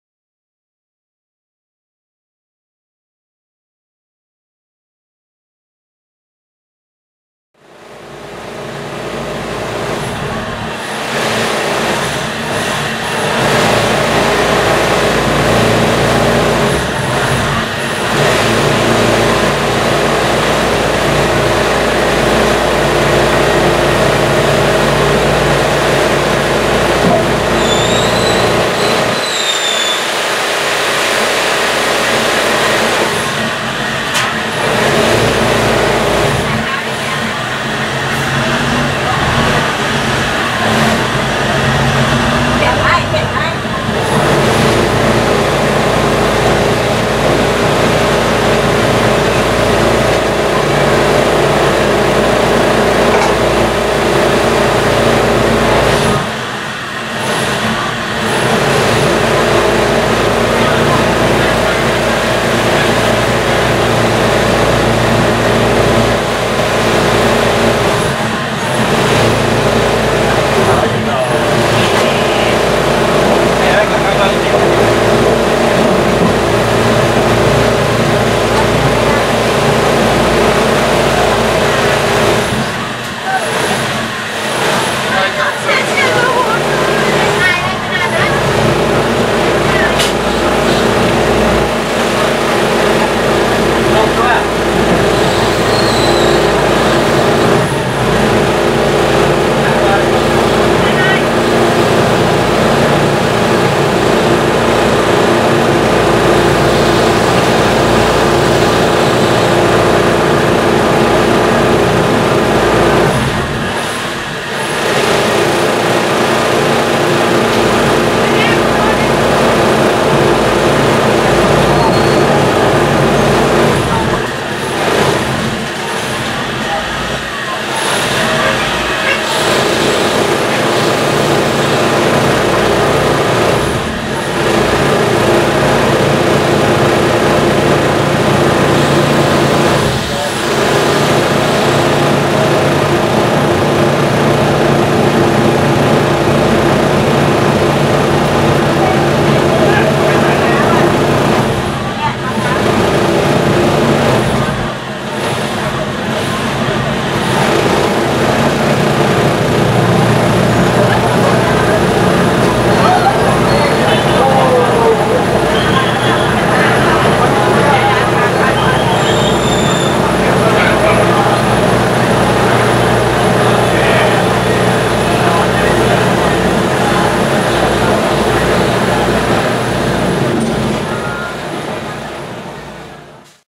【次々と線路上にテントが!? 】 タイの線路市場 後部展望 メークロン線 メークロン駅
メークロン線メークロン駅に到着する列車の最後部から撮った、タイの線路市場の様子です。